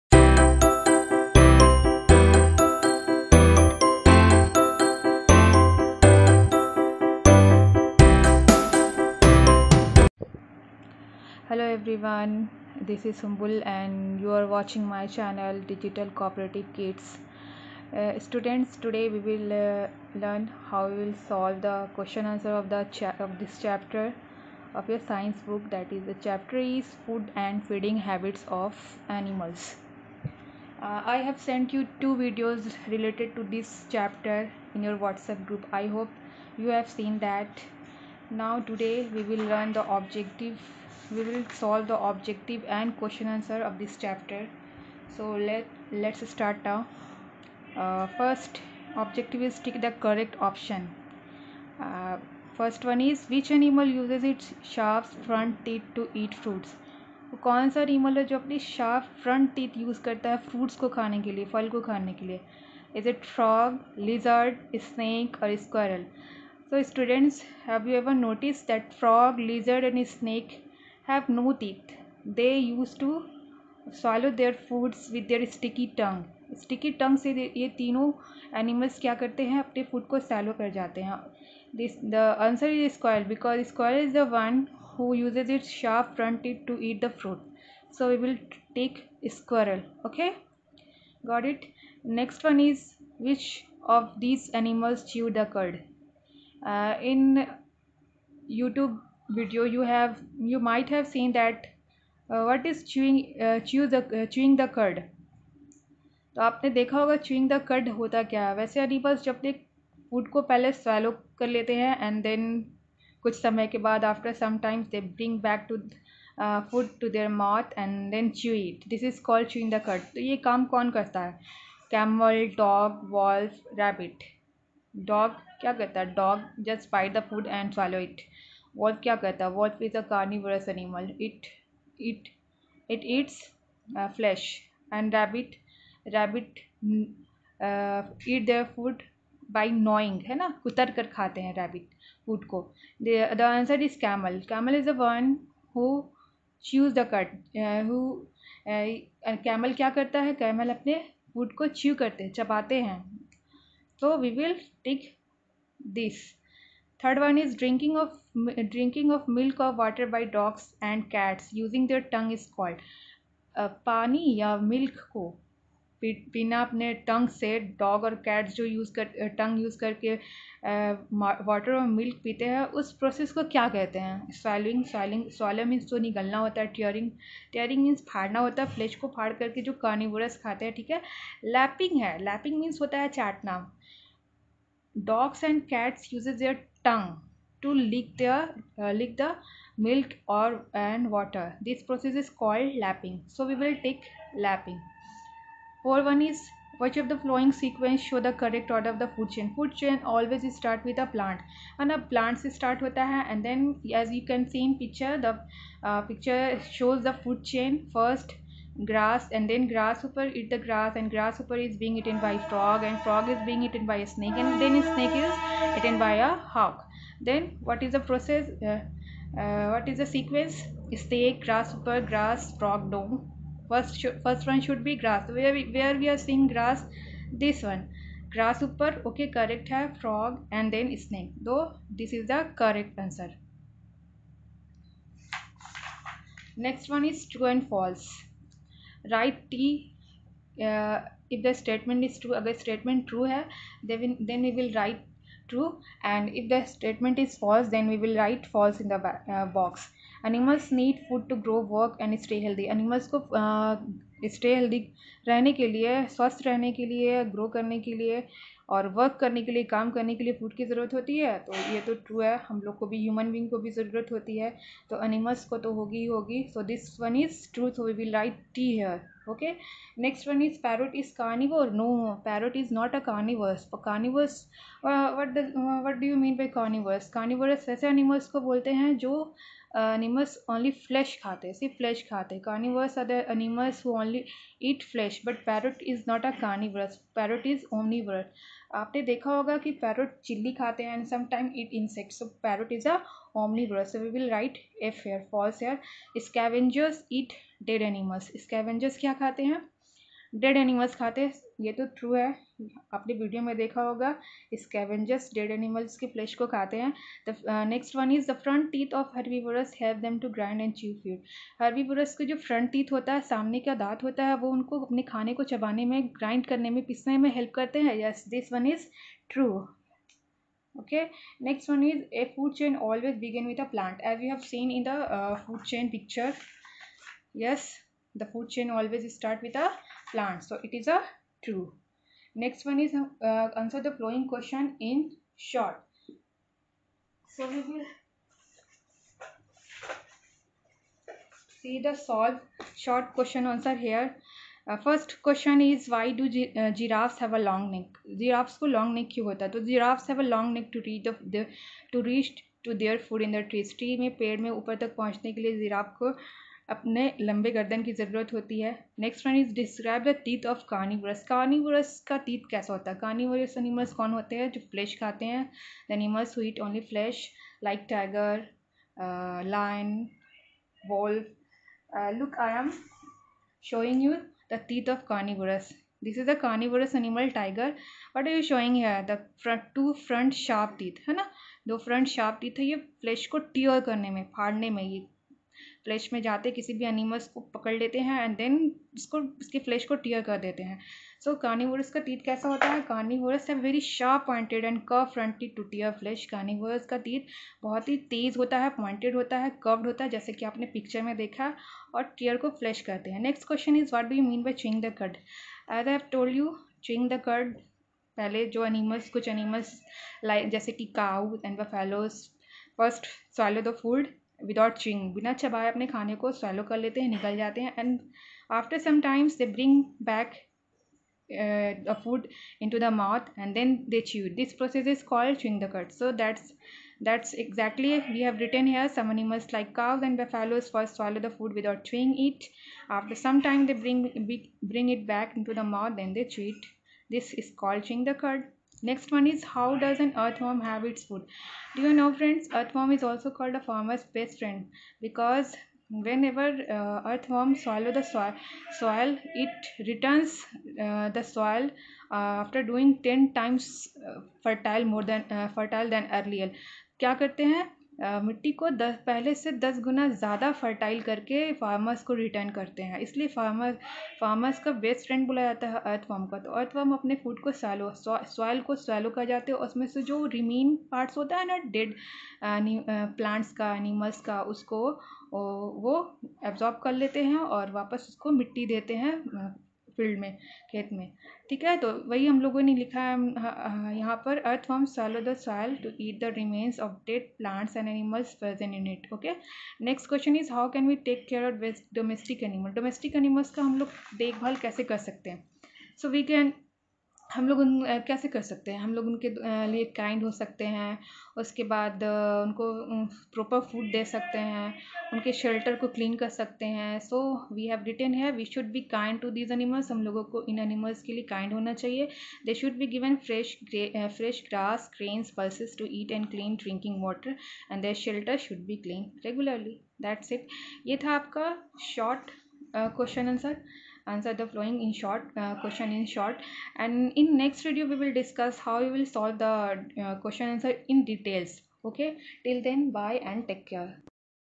Class 3 I Science I Chapter 1 (Food and Feeding habits of Animals)
Solved Question answer of chapter 1
# tick the correct options